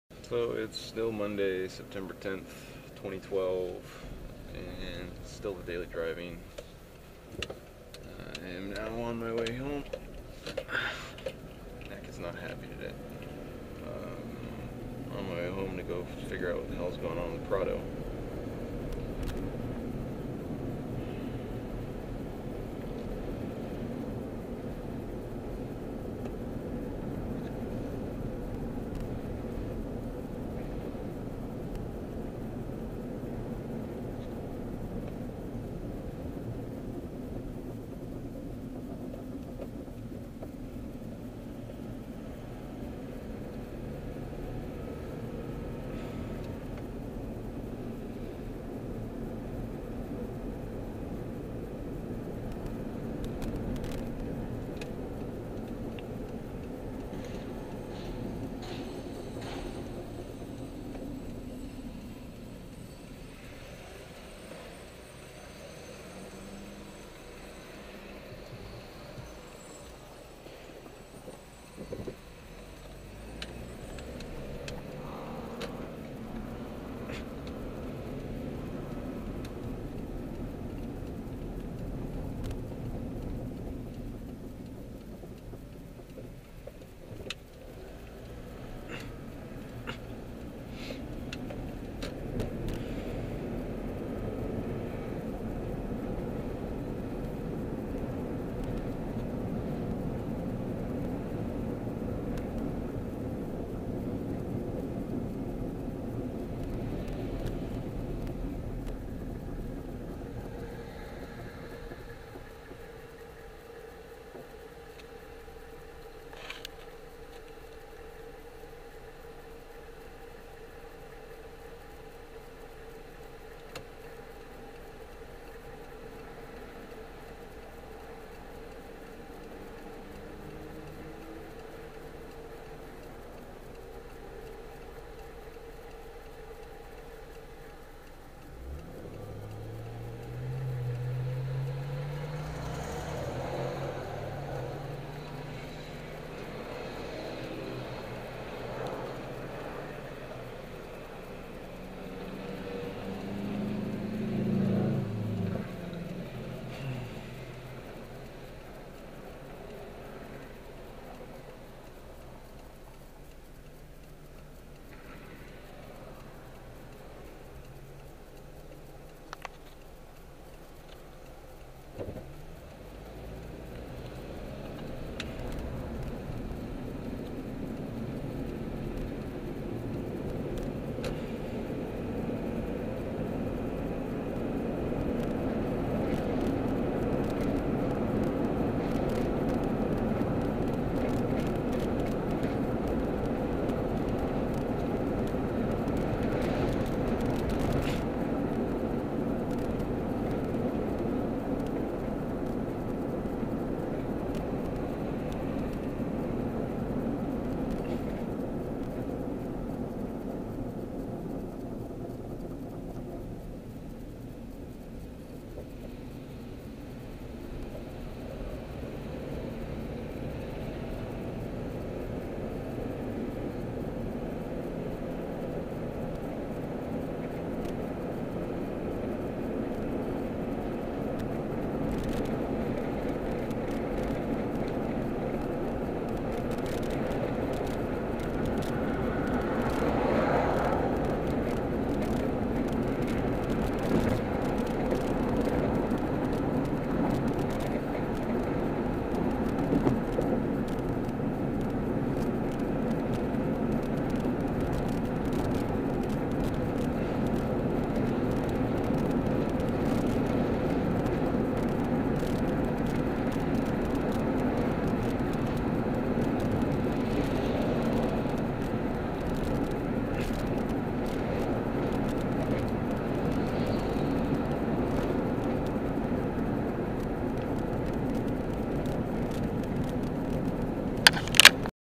2012.09.10 Daily Driving: Job, Home? [3/4]
Day three of the three-day notice I received on Friday. Stressed as fuck about Prado doing illegal stuff. Going to/from my current job. Driving around now with MORE commentary! Sometimes even special Guests. Just an example video from my Contour HD camera to show off the video quality to friends. You might want to turn down your speakers, the microphone is lacking.